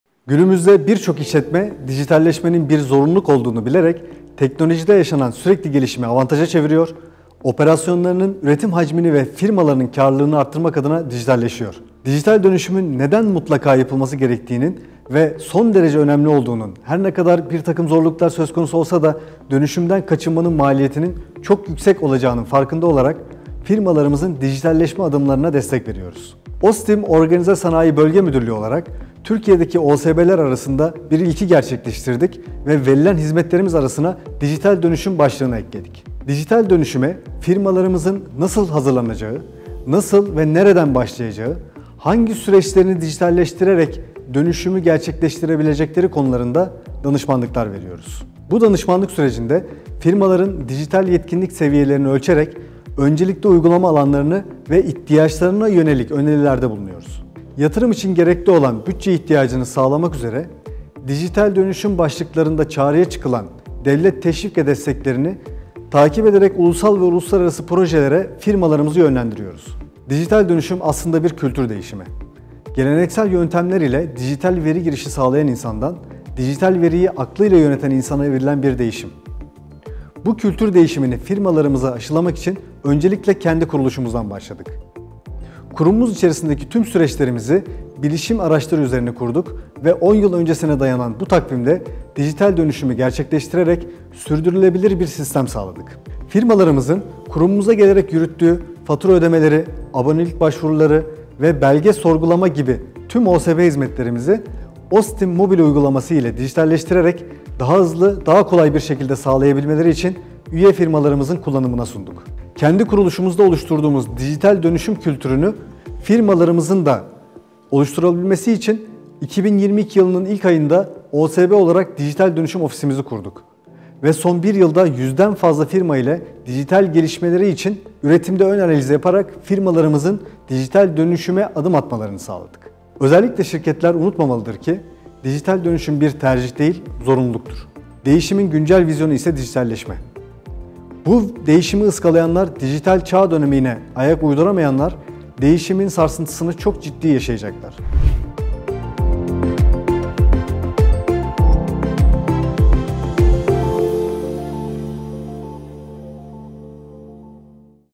Sanayide Dijital Dönüşüm | Uzman Görüşü
OSTİM’DE DİJİTAL DÖNÜŞÜM ÇALIŞMALARI
Günümüzde birçok işletme dijitalleşmenin bir zorunluluk olduğunu bilerek teknolojide yaşanan sürekli gelişimi avantaja çeviriyor, operasyonlarının üretim hacmini ve firmalarının kârlılığını artırmak adına dijitalleşiyor.
Dijital dönüşümün neden mutlaka yapılması gerektiğinin ve son derece önemli olduğunun, her ne kadar birtakım zorluklar söz konusu olsa da dönüşümden kaçınmanın maliyetinin çok yüksek olacağının farkında olarak, firmalarımızın dijitalleşme adımlarına destek veriyoruz.
OSTİM Organize Sanayi Bölge Müdürlüğü olarak, Türkiye’deki OSB’ler arasında bir ilki gerçekleştirdik ve verilen hizmetlerimiz arasına Dijital Dönüşüm başlığını ekledik.
• Dijital dönüşüme firmalarımızın nasıl hazırlanacağı, nasıl ve nereden başlayacağı, hangi süreçlerini dijitalleştirerek dönüşümü gerçekleştirebilecekleri konularında danışmanlık veriyoruz.
• Bu danışmanlık sürecinde, firmaların dijital yetkinlik seviyelerini ölçerek öncelikli uygulama alanlarına ve ihtiyaçlarına yönelik önerilerde bulunuyoruz.
• Yatırım için gerekli olan bütçe ihtiyacını sağlamak üzere dijital dönüşüm başlıklarında çağrıya çıkılan devlet teşvik ve desteklerini takip ederek ulusal ve uluslararası projelere firmalarımızı yönlendiriyoruz.
Dijital Dönüşüm aslında bir kültür değişimi. Geleneksel yöntemler ile dijital veri girişi sağlayan insandan, dijital veriyi aklıyla yöneten insana evrilen bir değişim. Bu kültür değişimini firmalarımıza aşılamak için öncelikle kendi kuruluşumuzdan başladık.
Kurumumuz içerisindeki tüm süreçlerimizi bilişim araçları üzerine kurduk ve 10 yıl öncesine dayanan bu takvimde dijital dönüşümü gerçekleştirerek, sürdürülebilir bir sistem sağladık.
Firmalarımızın kurumumuza gelerek yürüttüğü fatura ödemeleri, abonelik başvuruları ve belge sorgulama gibi tüm OSB hizmetlerimizi, Ostim mobil uygulaması ile dijitalleştirerek daha hızlı, daha kolay bir şekilde sağlayabilmeleri için üye firmalarımızın kullanımına sunduk.
Kendi kuruluşumuzda oluşturduğumuz dijital dönüşüm kültürünü firmalarımızın da oluşturabilmesi için 2022 yılının ilk ayında OSB olarak Dijital Dönüşüm Ofisimizi kurduk ve Son 1 yılda 100’den fazla firma ile dijital gelişimleri için üretimde ön analiz yaparak firmalarımızın dijital dönüşüme adım atmalarını sağladık.
Özellikle şirketler unutmamalıdır ki, dijital dönüşüm bir tercih değil, zorunluluktur. Değişimin güncel vizyonu ise dijitalleşme. Bu değişimi ıskalayanlar, dijital çağ dönemine ayak uyduramayanlar değişimin sarsıntısını çok ciddi yaşayacaklar.
Biz de Ostim Dijital Dönüşüm Ofisi olarak, tüm bölge ekosistemimizdeki firmalarımızın bu dijital dönüşüm yolculuğunda yanındayız ve faaliyetlerimizi sürdürmeye devam edeceğiz.